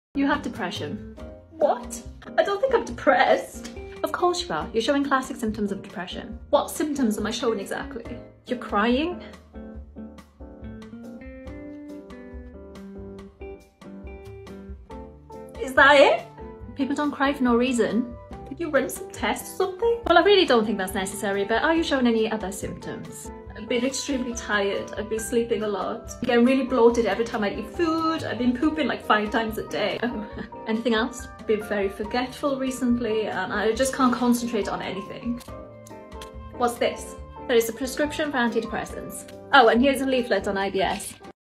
UK healthcare in a nutshell
I wish doctors would listen to their patients more! It took years for me to get diagnosed with coeliac disease because no doctor would take my symptoms seriously.